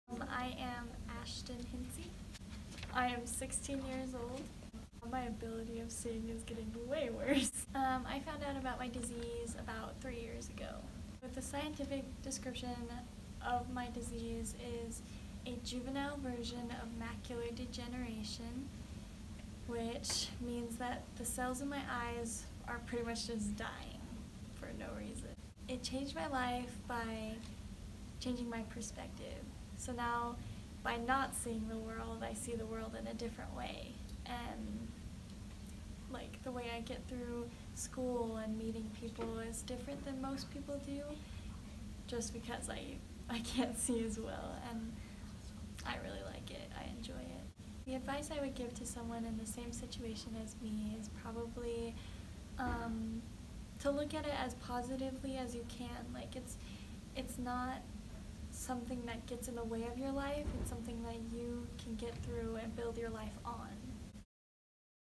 One Minute Profile: Ashton HIntze
11th Annual CSD Film Festival
High School Documentary
by Thaysa Gomes-Evangelista, Martina Lomen - Brighton High School